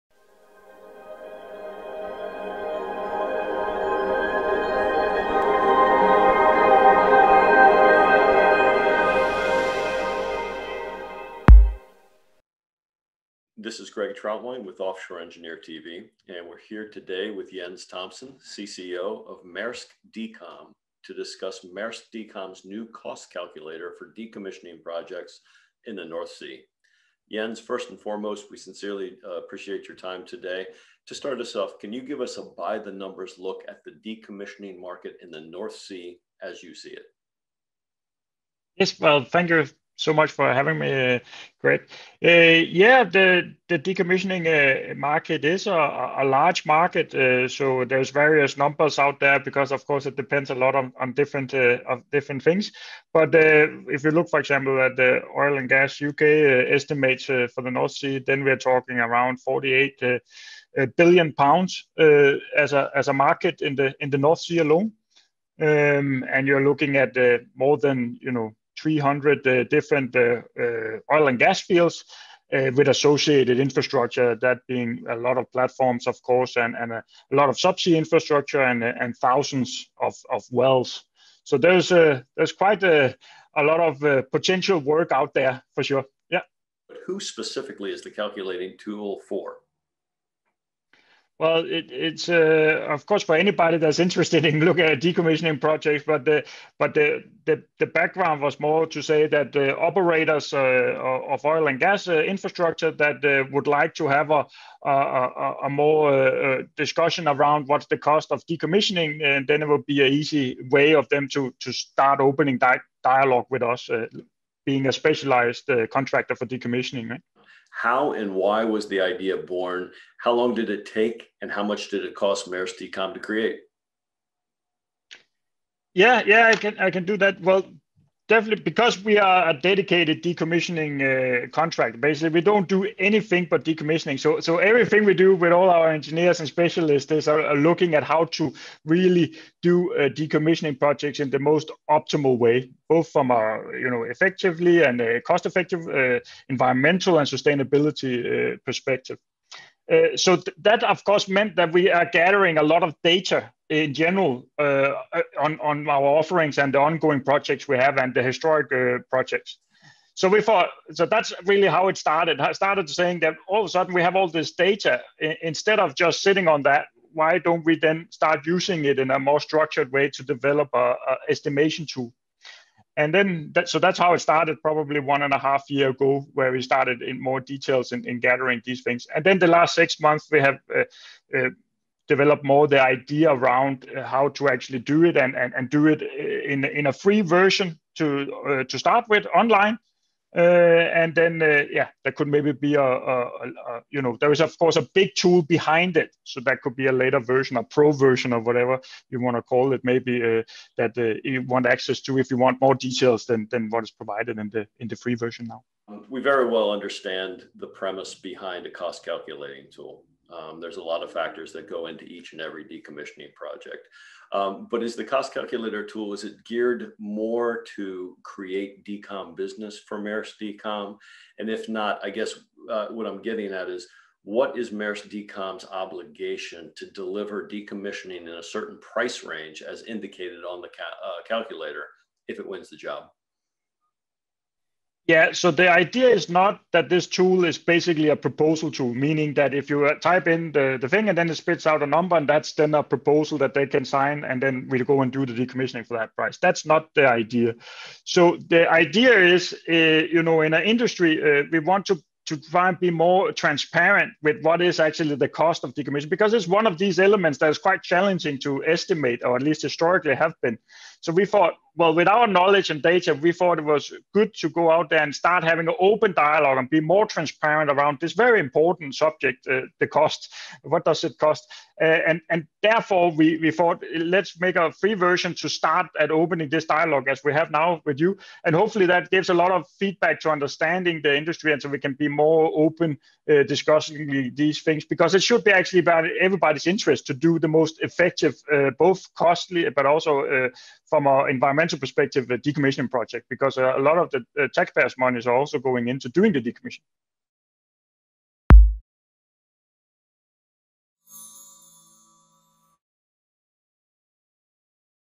Offshore Decommissioning: Calculating the Costs
Maersk Decom designed a new cost calculating tool for benchmarking decommissioning projects in the North Sea. Jens K. Thomsen, CCO, Maersk Decom, discusses with OE TV.

Thumbnail photo credit: Lukas Z/AdobeStock